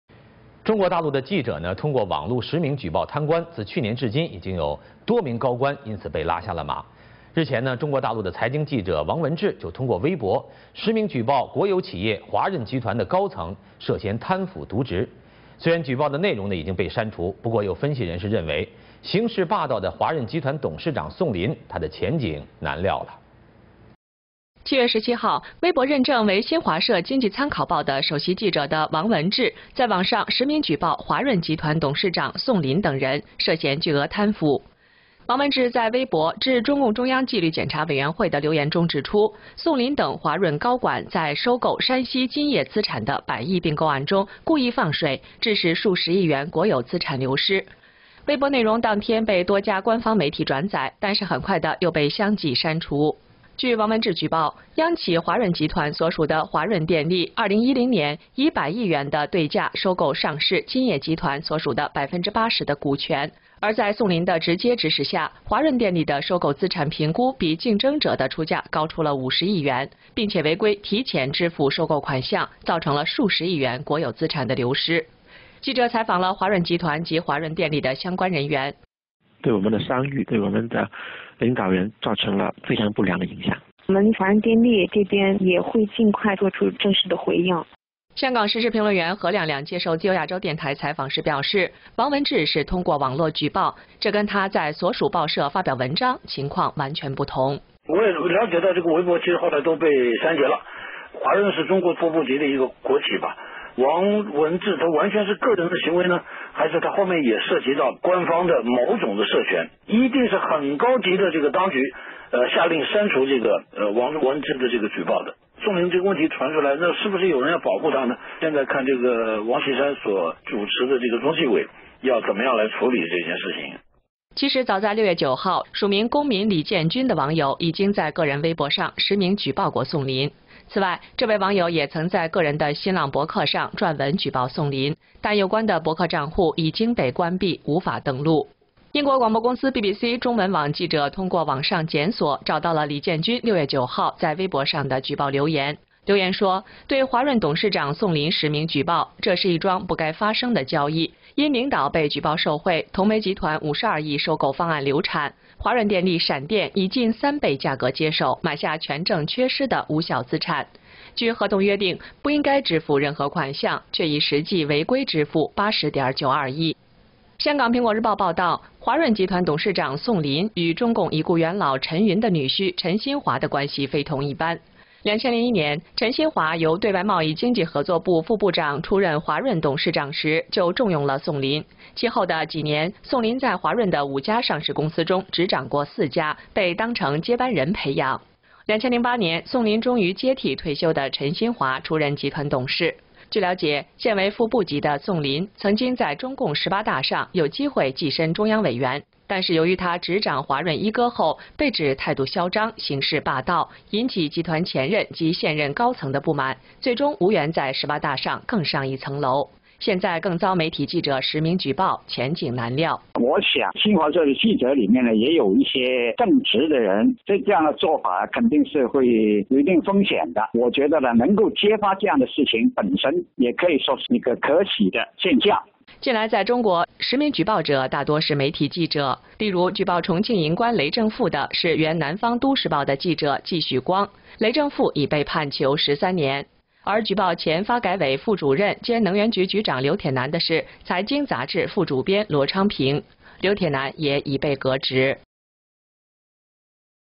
Chinese Reporters Expose Corruption Using Their Real Names
Since last year, Mainland Chinese reporters have exposed and
taken down many corrupt officials, including those at high levels.
Recently, Wang Wenzhi, a Mainland Chinese reporter for Caijing
(Finance magazine), exposed a high level official
of state-owned enterprise., "China Resources Company"
(CRC) who is involved in corruption.
Although the reported content has been deleted,
many analysts say that prospects for overbearing chairman
of CRC, Song Lin, are unpredictable.

On July 17, Wang Wenzhi, certified on his Weibo microblog as
"Xinhua News Agency, Economic Information Daily Chief Reporter",
posted online using his real name that Song Lin, chairman
of CRC, and others are involved in massive corruption.
Wang said in his weibo post titled, "To the CCP Central
Discipline Inspection Commission," that Song
and other CRC high officials intentionally lost billions of
state assets in the purchase of the Shanxi company "Jinye".
Wang's post was forwarded by many official media on
the same day; however, it soon removed.

According to Wang, China Resources Power, a subsidiary of
state-owned enterprise CRC, purchased 80% stake of "Jinye Group"
for ten billion yuan in 2010.

However, under Song Lin's instruction, CRC's acquisition
of assets evaluation was 5 billion yuan ($814 million) .
It is more than the competitor's bid, and was illegally
paid in advance, which results in a loss of billions of state assets.

NTD reporter spoke to related personnel
at CRC and CR Power.

Mr. Liang of CRC: "This has created a very bad image
for our reputation and our leaders."

Ms. Du, CR Power office clerk:
"We at CR Power will soon give a formal response."

Hong Kong commentator He Liangliang told Radio Free Asia
that Wang Wenzhi posted the reported on the internet,
which is entirely different from the publishing articles in his
newspaper office.

He Liangliang, Hong Kong commentator: "I've learned that
his post was later removed from weibo.
CRC is a vice-ministerial level state-owned enterprise.

Is it Wang Wenzhi's personal action,
or does it relate to some kind of official authority behind him?
It must be a very high level authority that ordered to
remove Wang's report.
Is it someone who wants to protect Song Lin?

Now we need to see how the Central Discipline Inspection
Commission, headed by Wang Qishan, will deal with this matter."

Actually, on June 9, a netizen pen named "Citizen Li Jianjun,"
had reported Song Lin on his personal weibo account.
In addition, this netizen had also previously posted articles
denouncing Song Lin, but the related account was closed.

A reporter for BBC's Chinese language website found, via
a web search, a message from Li Jianjun on June 9.
It said, "Regarding the report on CRC chairman Song Lin with
my real name: This is a transaction which should not happen—
because the closing of acquisition project
of "Datong Coal Mine Group",
CRC suddenly purchased invalid assets which missing
warrants for three times the price;
according to the contract, should not pay any money,
however, they have actually paid 8 billion ($1.3 billion) illegally."

Hong Kong's Apple Daily reported that CRC chairman,
Song Lin, has an unusual relationship with
former Chinese Communist Party leader
Chen Yun's son in law Chen Xinhua.
In 2001, as a chairman of CRC, Chen Xinhua
promoted Song Lin.
In the following years, Song Lin was seen as the upbringing
successor and governed four of the the CRC's five listed companies.
In 2008, Song Lin finally replaced Chen Xihua
as chairman of the group.

It's known that since Song Lin is at the vice-ministerial level,
he's had the chance to become a Central Committee in the 18th Congress,
however, since he took over CRC, he was criticized as
too arrogant and overbearing,
which arouses dissatisfaction among current high level
officials, so he couldn't get into the 18th Congress.
Now that he's been exposed by reporters
using their real names, his prospects are unpredictable.

Zhang Chengjue, Hong Kong freelance writer: "I think there
could have some honest reporters in the "Xinhua News Agency", but such an action has it's risks.
I think the fact he's brave enough to expose it is a good sign."

Lately, those in China doing reports with their real names
are mostly media reporters.
For example, Ji Xuguang, former "Southern Metropolis Daily"
reporter, exposed the sex scandal of Chongqing official, Lei Zhengfu, who was sentenced to 13 years.
Luo Changping, deputy editor of Caijing, exposed former
National Development and Reform Commission deputy
director and Energy Bureau secretary Liu Tienan.
Liu Tienan has now been dismissed.